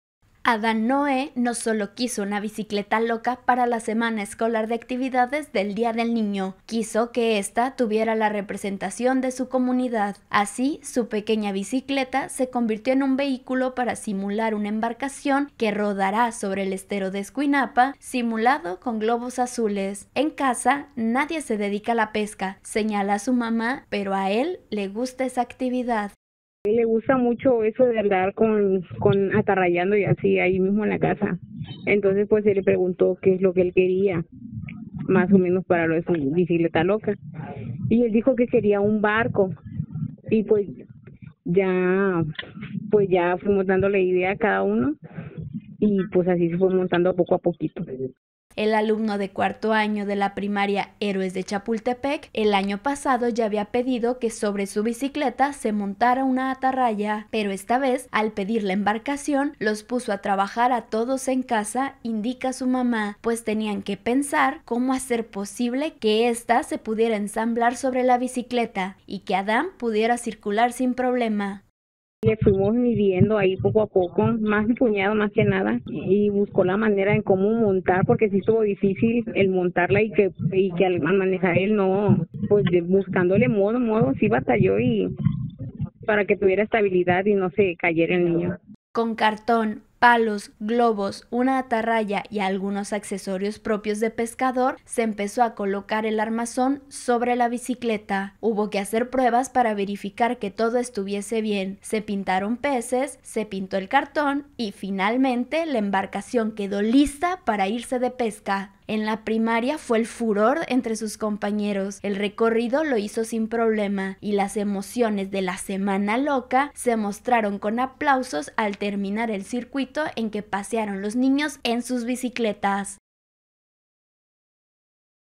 Adán Noé rinde homenaje a la bicicleta y la pesca de Escuinapa
Pidió a su familia una ‘bicicleta loca’ como parte de la semana escolar de actividades del Día del Niño y de la Niña.